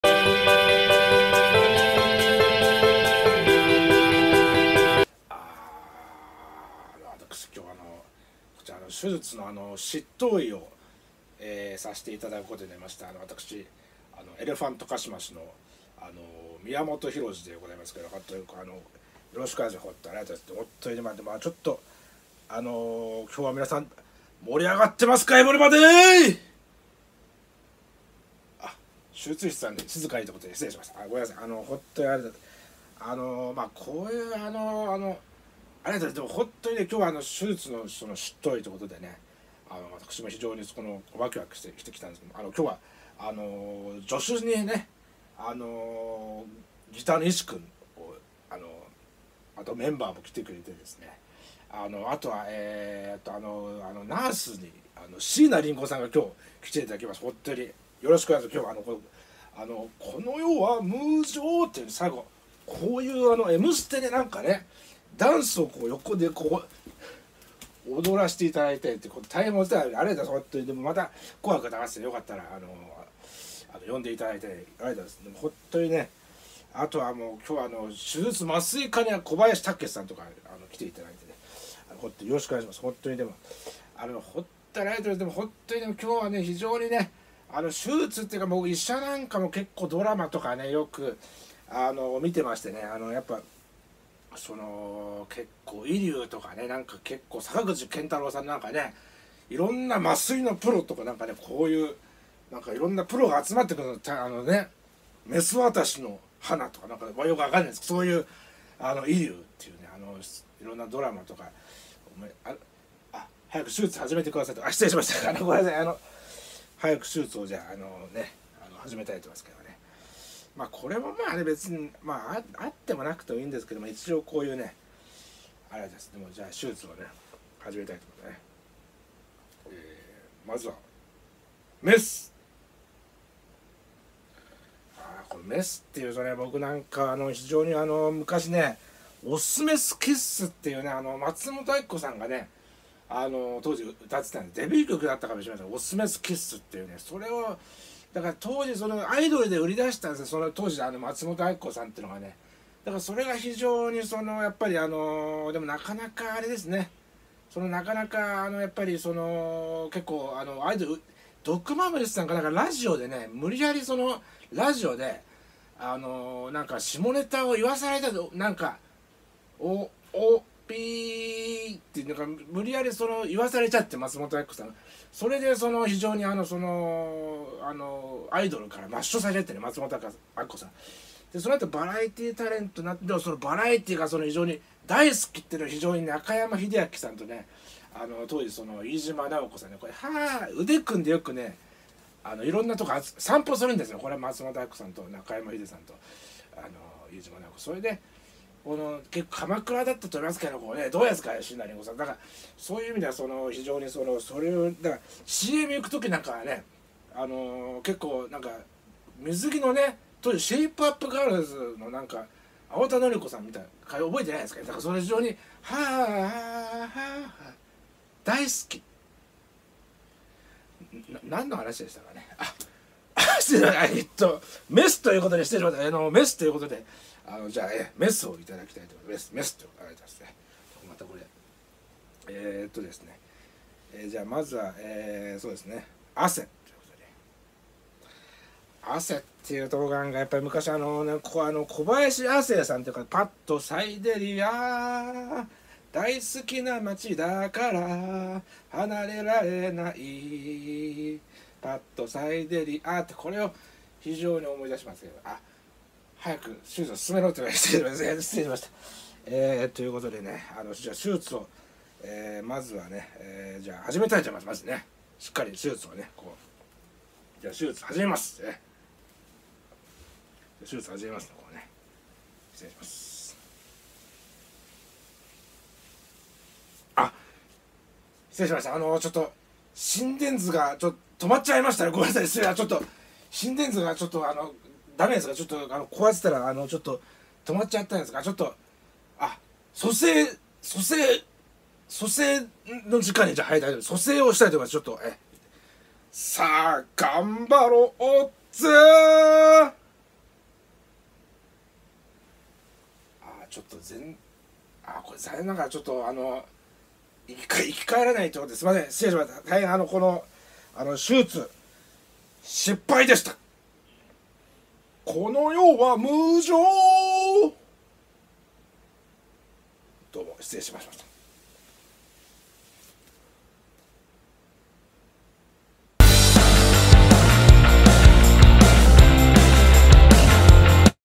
はなちゃんねるのエレカシ宮本コント「執刀医」
エレファントカシマシ宮本浩次さんによるコント、手術の執刀医です！
医療ドラマばかり見ていたら出来たネタです。
果たして手術を無事成功することが出来るのか、乞うご期待ください！